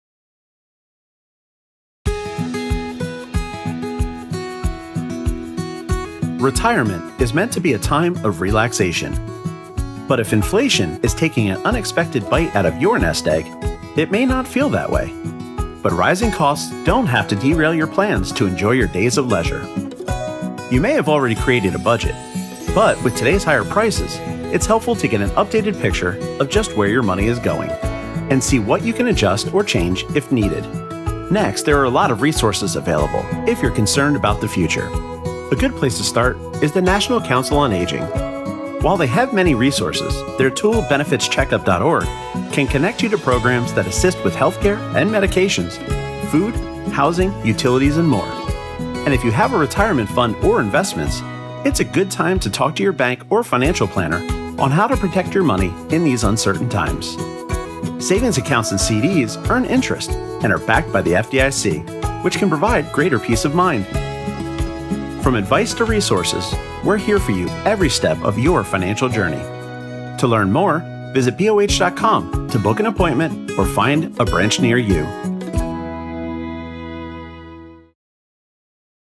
Surviving Inflation on a Fixed Income
Staying within your budget can feel impossible when you’re facing inflation on a fixed income. But, it doesn’t have to be. In this video, we share tips and resources that can help you keep your finances on track. For more financial tips and resources, visit our